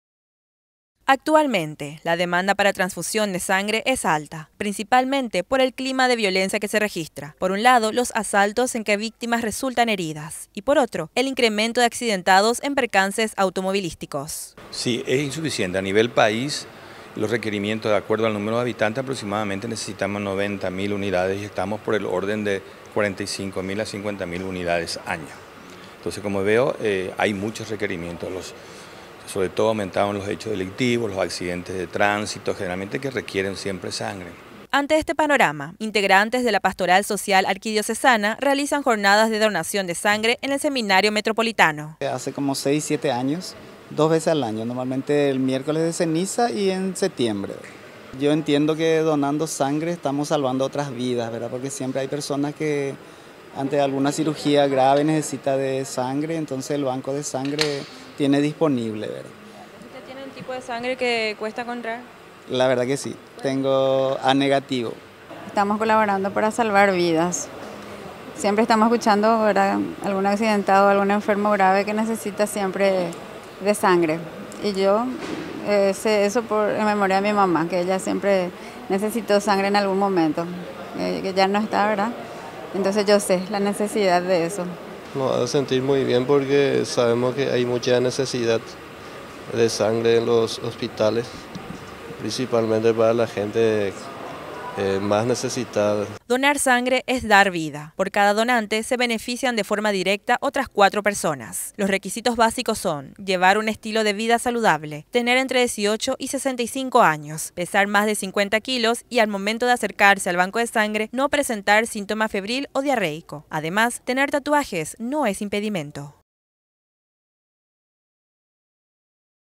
Sangre donada no cubre alta demanda
Apenas la mitad de la demanda de sangre se cubre con la donación que se realiza de forma voluntaria. Se requiere unas 90 mil unidades, lo cual motiva a diversos grupos para ayudar a los demás.